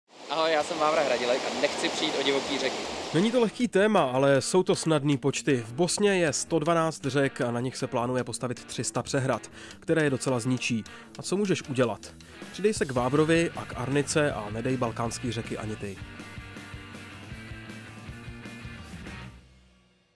BALKÁNSKÉ ŘEKY NEDÁME! Vavřinec Hradilek a snadné počty
Buď jako Vávra Hradílek a pomoz zachránit divokou přírodu!
„Řeky a příroda vůbec je na Balkáně úžasná a přitom je to pro nás jen pár hodin jízdy. Nemůžeme se tvářit, že se nás to nedotýká,“ říká Vavřinec Hradilek.

Trojnásobný mistr světa a stříbrný olympionik z her v Londýně chce přispět k záchraně řek na Balkáně, které ve velkém mizí kvůli výstavbě vodních elektráren. Celá řada z nich přitom porušuje stavební nebo ekologické předpisy.

V Arnice jsme se proto rozhodli spolu s bosenským Centrem za životní prostředí rozjet akci na záchranu této výjimečné přírody a děkujeme Vávrovi, že se k nám připojil.